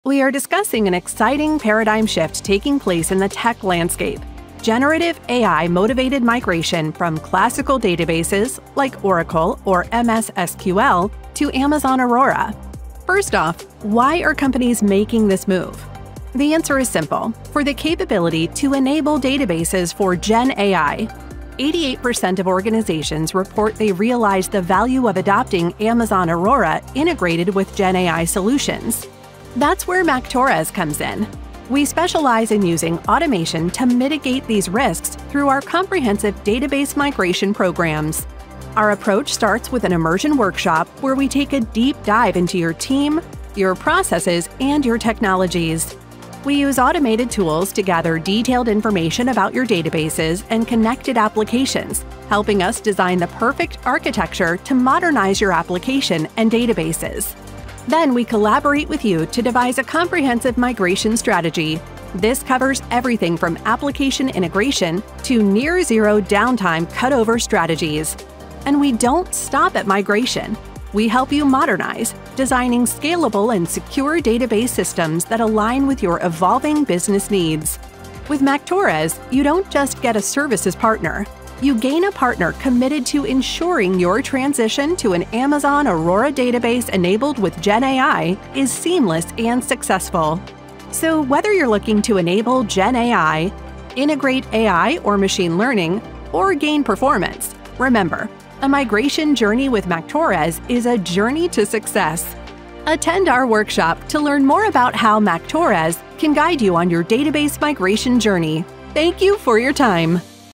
Enable Gen AI with Amazon Aurora by Automating Database Migration
Are you among the 90% challenged by where to start your GenAI modernization journey? Join us in a transformative workshop explicitly designed for technical leaders navigating the complex terrain of adopting cloud-native databases for GenAI. Experience the automation that guides data and application code migration tasks, and see how you can streamline and de-risk your migration journey.

You can take advantage of this unique opportunity to learn from industry leaders and gain hands-on experience.